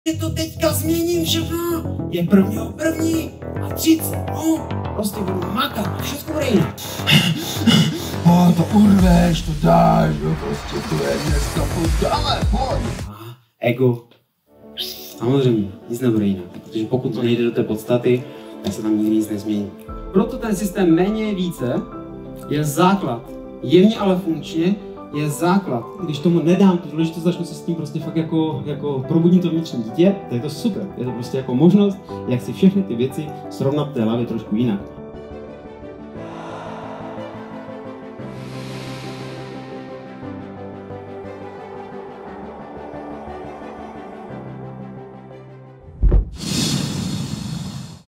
Jak ti bude v životě lépe? Na konci 1 minuty máš jasno :-)
Když MÉNĚ ZAČNE BÝT VÍCE - Dýchánky 45 - Proč se vyplatí žít v duchu "Méně je více"
Je radost dělat radost, Rosťa :-)